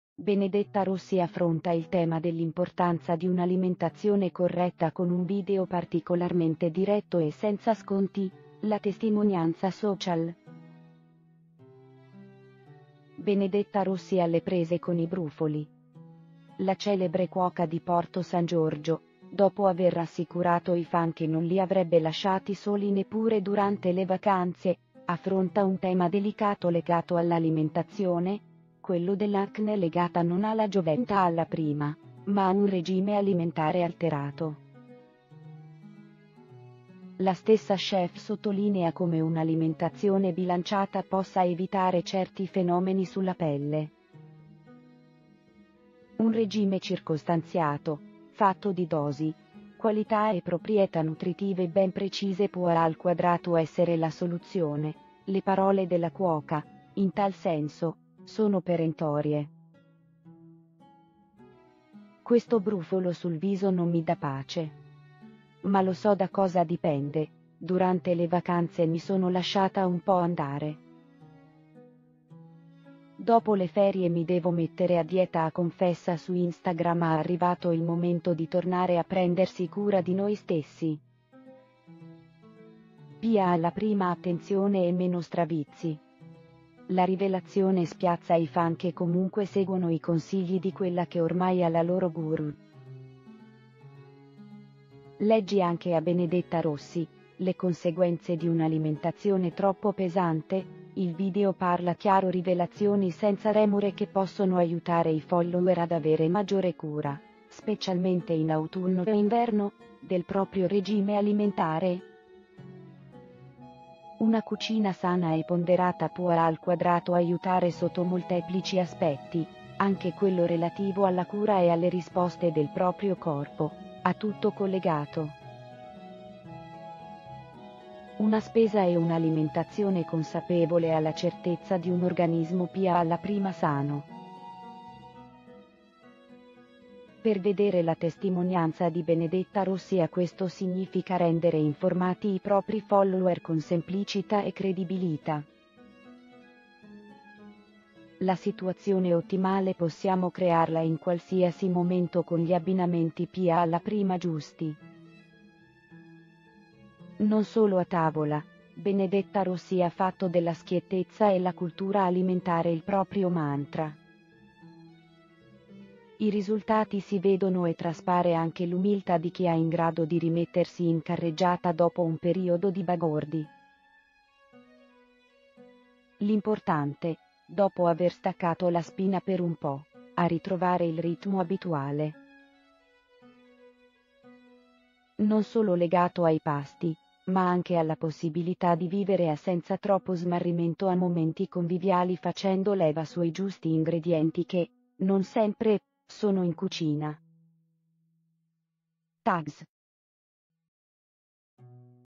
Benedetta Rossi, la confessione ai fan: il dettaglio non passa inosservato
Benedetta Rossi affronta il tema dell’importanza di un’alimentazione corretta con un video particolarmente diretto e senza sconti: la testimonianza social.   Benedetta Rossi alle prese con i brufoli.